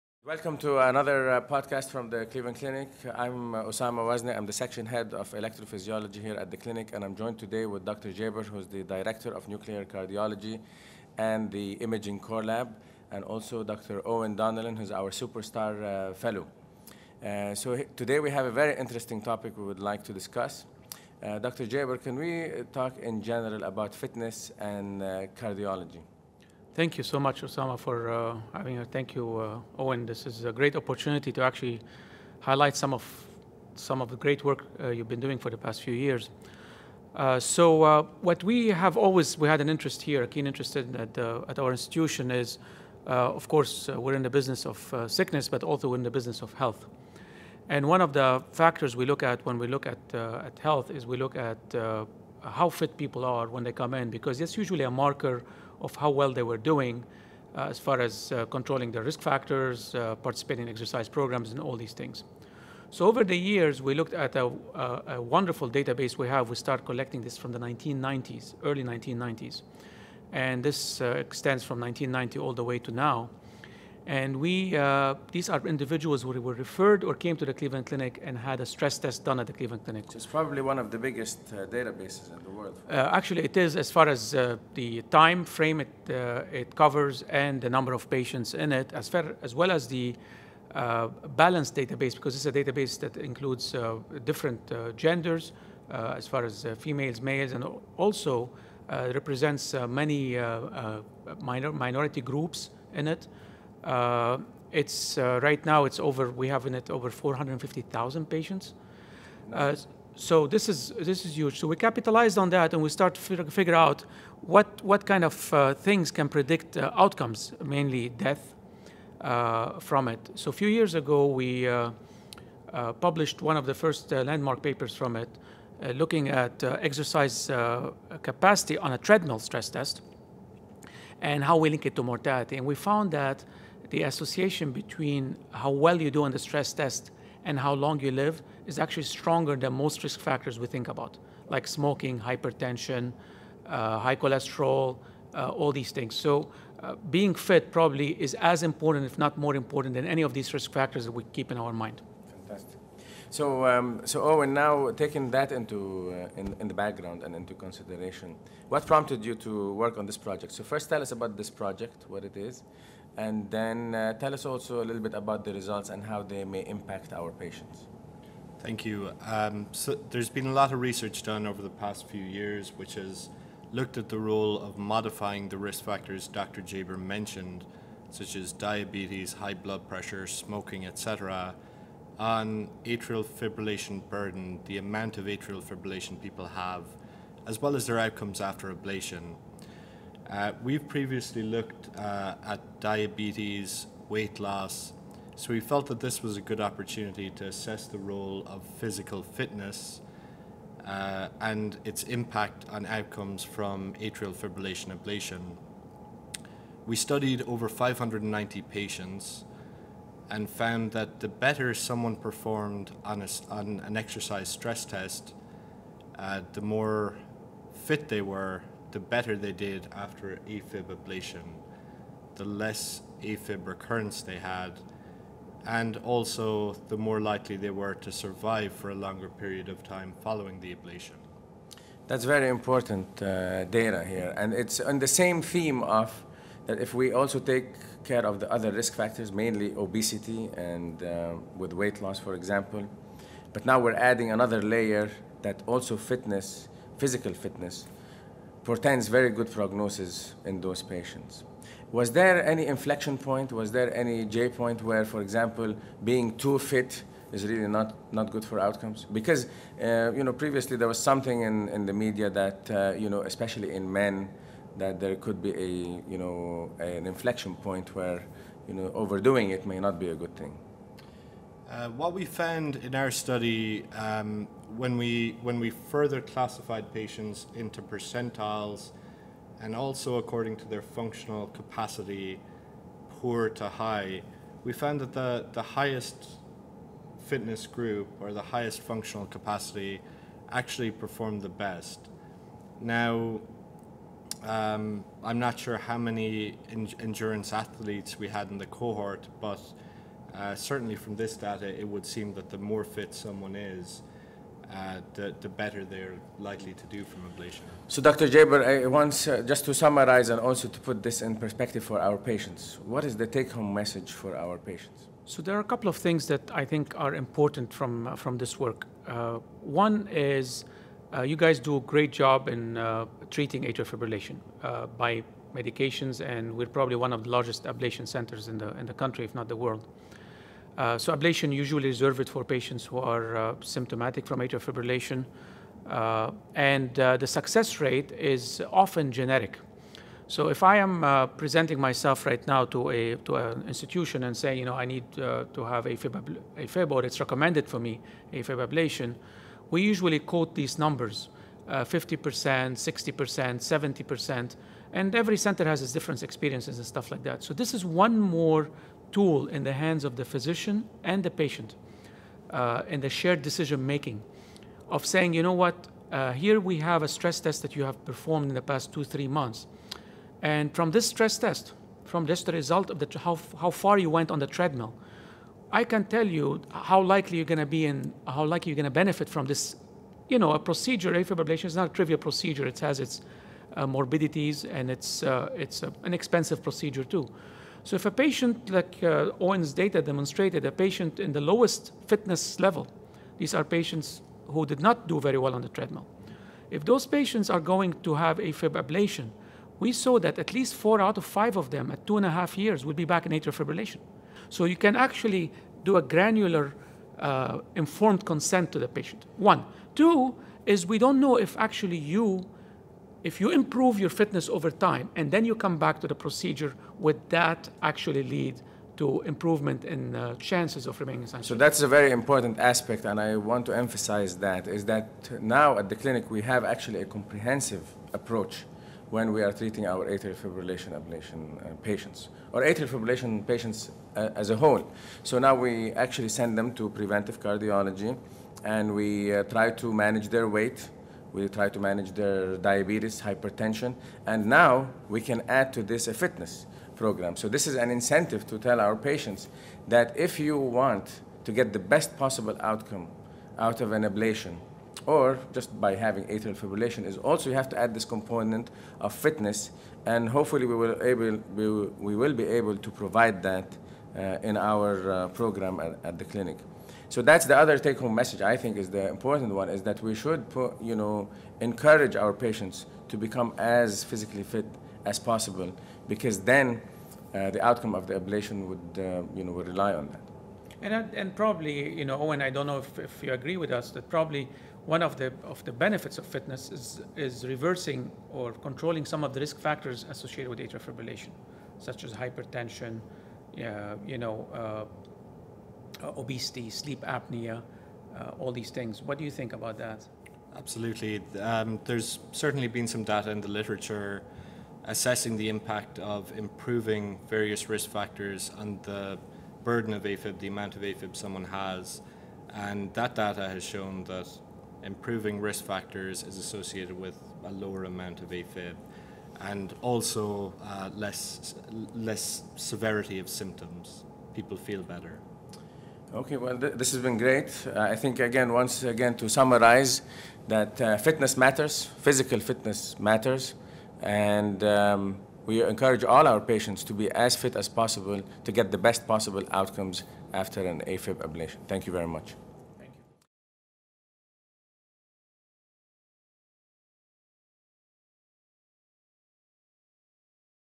Fitness Matters When It Comes to Atrial Fibrillation Treatment
When being treated for atrial fibrillation, being fit is best. Investigators found that those with the best cardiovascular fitness more likely to maintain long term sinus rhythm, come off antiarrhythmic medications and have improved mortality. This study reinforces the impact that cardiovascular risk factors such as weight, blood pressure, diabetes, sleep apnea - and now cardiovascular fitness has on atrial fibrillation management.

Oussama Wazni, MD, Section Head Electrophysiology and Pacing discusses this exciting paper with lead author Eoin Donnellan, MD and Wael A. Jaber, MD